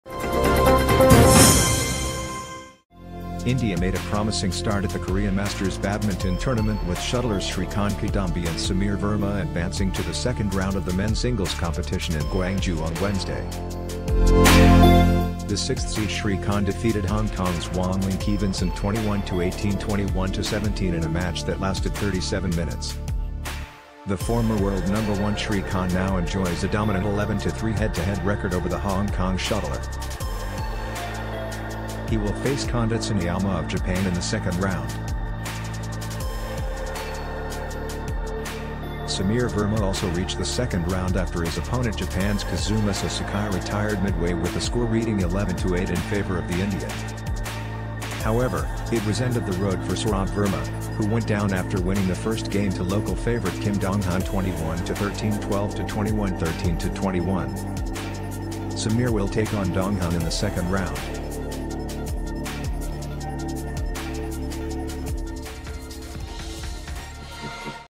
Korea Open: Srikanth, Sameer enter round 2; Sourabh bows out
India made a promising start at the Korea Masters badminton tournament with shuttlers Srikanth Kidambi and Sameer Verma advancing to the second round of the men’s singles competition in Gwangju on Wednesday.